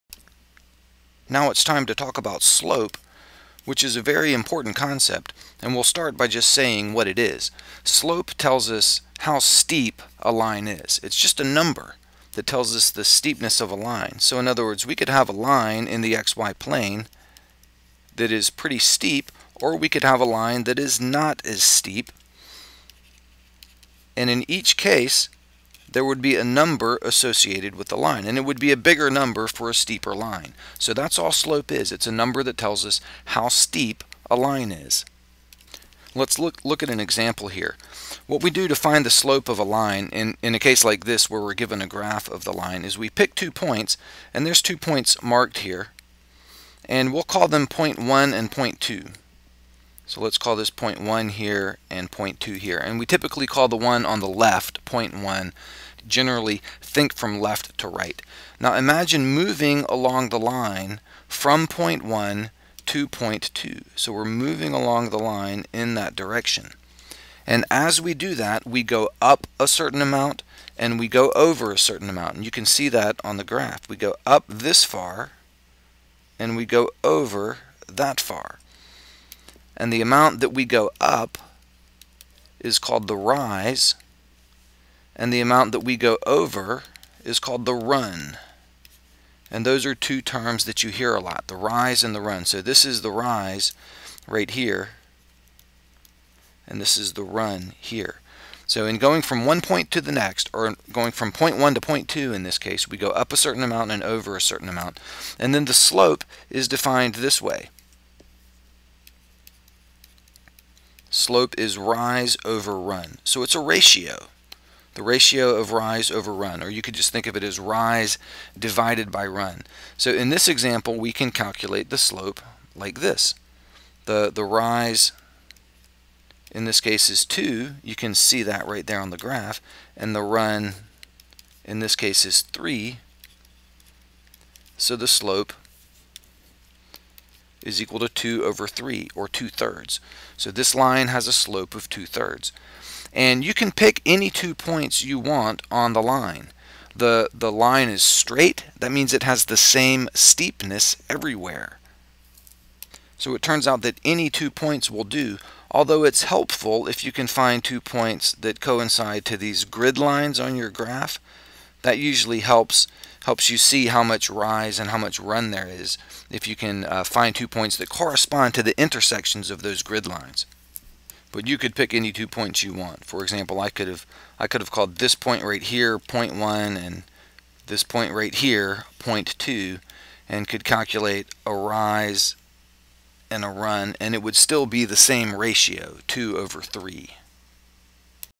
Algebra 1 8.04a - Slope
An introduction to the concept of slope, explained from the ground up, both conceptually and numerically.  (This video has been re-uploaded to fix an error.)